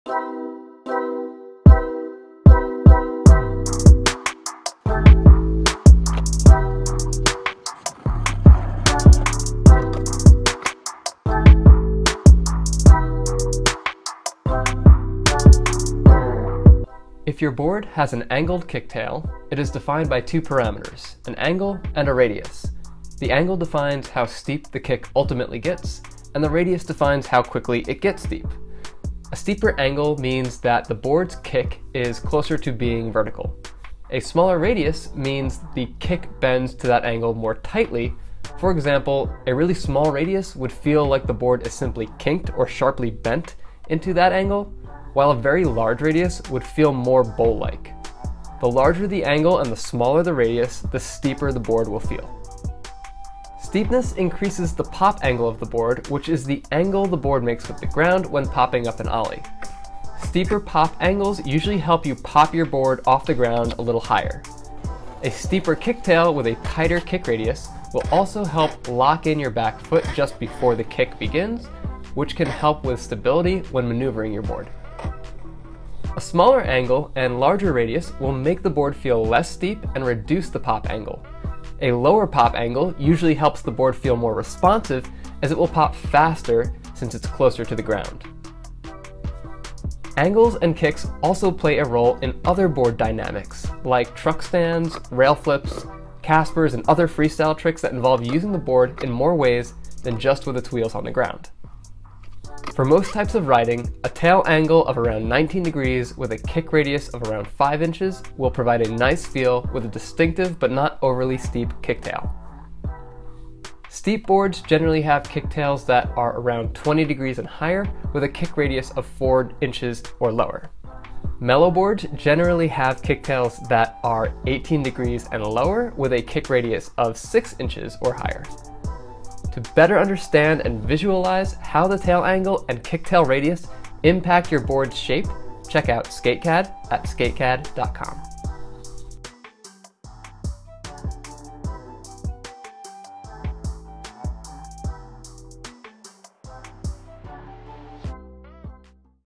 Skateboard Design: Tail Angle and Radius
0:00 Intro
0:17 What is tail angle and tail radius
1:00 More vs. less concave
1:43 Beginner tip
2:09 Tail angle and radius selection flow chart

This video covers skateboard and longboard tail angle and tail radius, and how to select those characteristics for your board. You can design your own boards and download the files for making them in SK8CAD

*Small error: at 1:30, I say "angles and kicks" when I meant to say "kick angles and radii". Hopefully the intent of my comment was still clear :) .